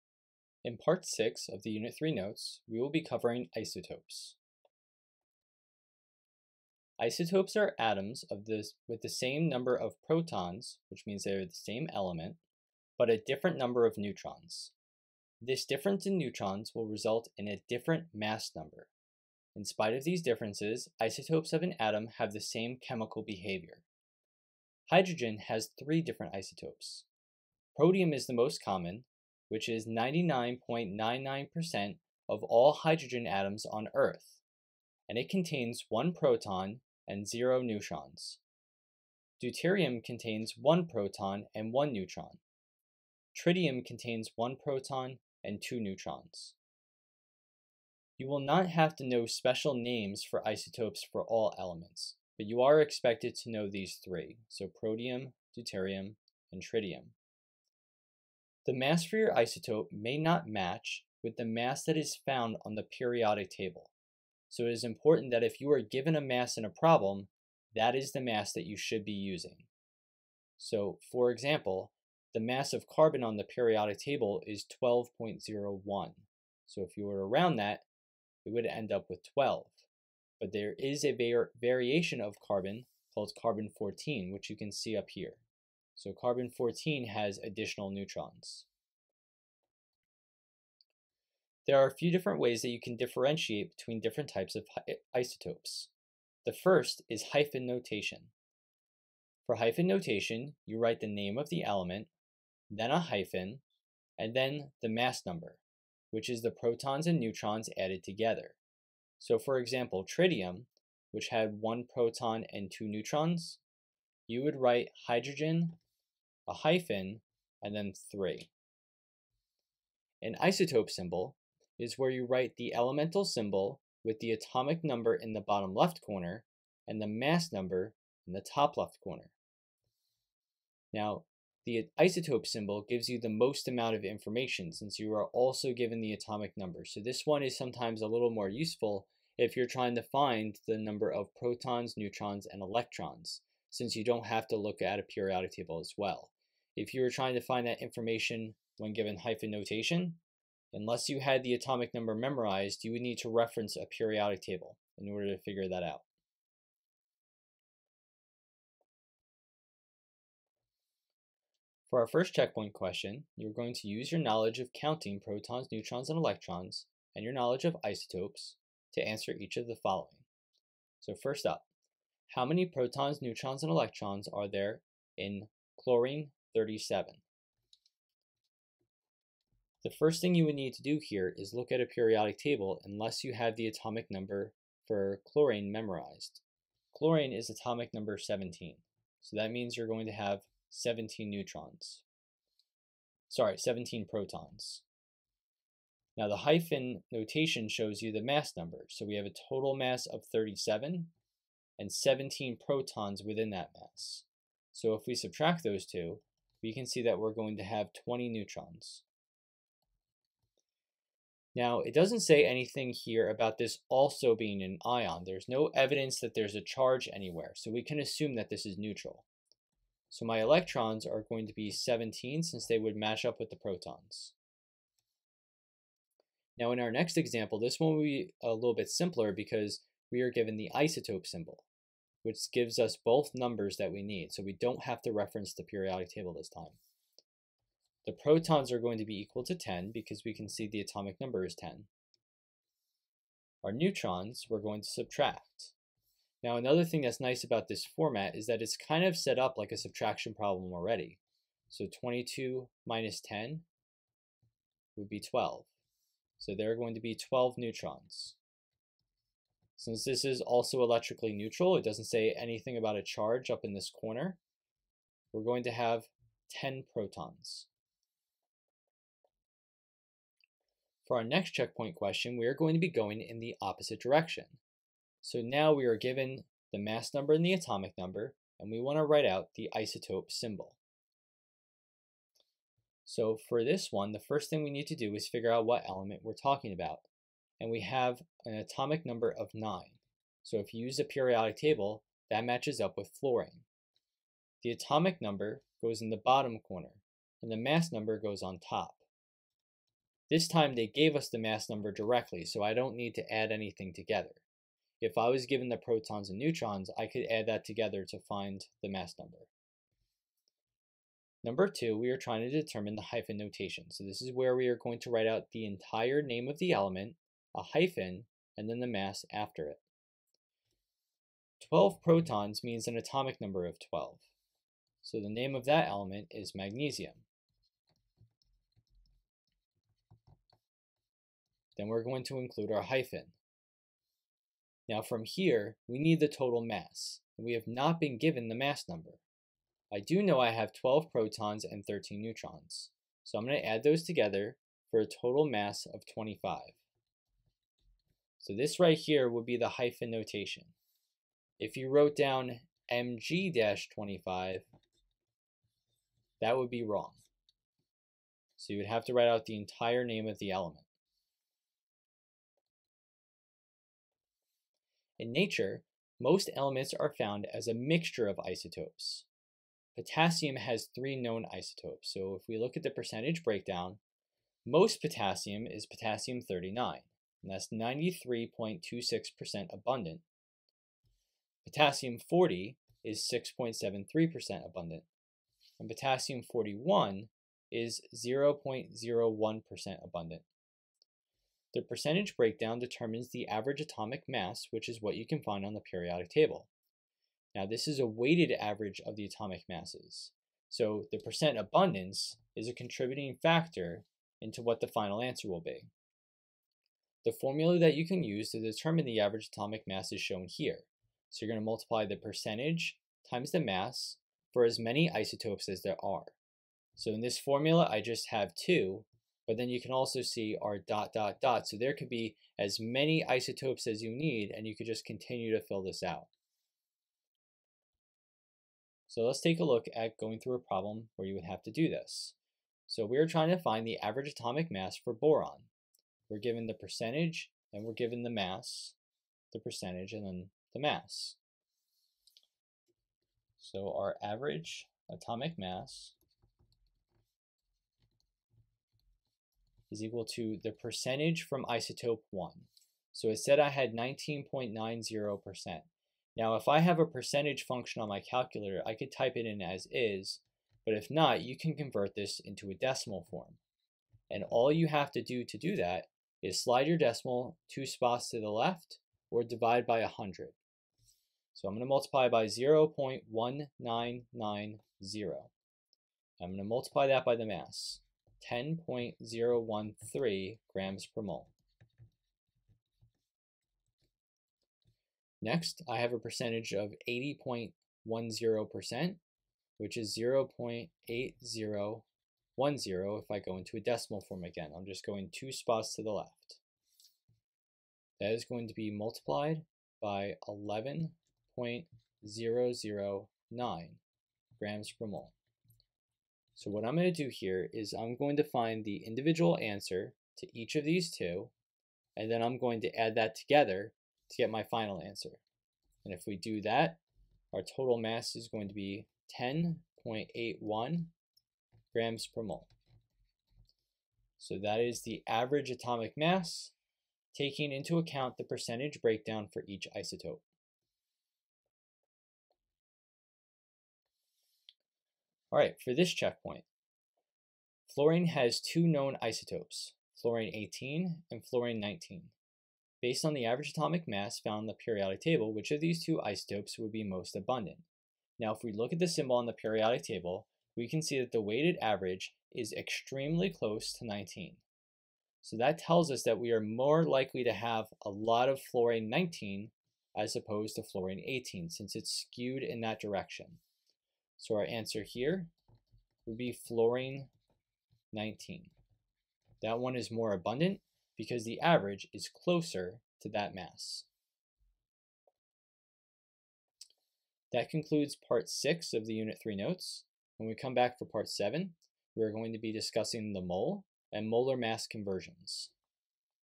Unit 3 Notes Part 6
This is part 6 of 8 for the Unit 3 notes. Topics covered include isotopes.